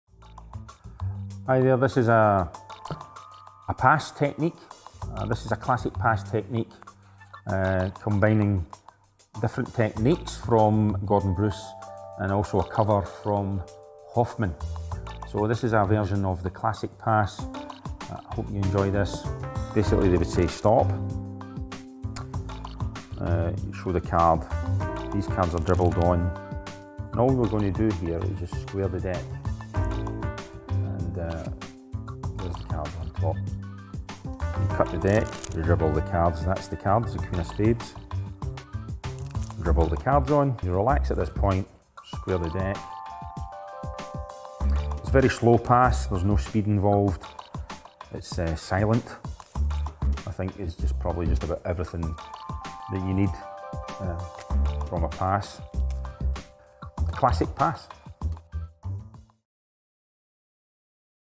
Eddie McColl Pass Trailer
In this download, Eddie covers his handling of the Classic Pass. Eddie has spent years combining several different techniques from Derek Dingle, Hoffman, Gordon Bruce and Roy Walton to create a pass that is very slow and silent. Everything you need in a classic pass.
As well as detailing the Classic Pass, in this download, Eddie touches on a tip from Roy Walton regarding having a continuing action following a sleight. This idea takes a sleight from being a move to being part of something else, which acts to hide the work.

“As with Eddie’s DVD’s, these specialized videos will take your handling to the next level. Eddie is a real pro and we are lucky that he has chosen to share his thoughts and methods with the magic community. You should take advantage of his generosity.” – Gary Plants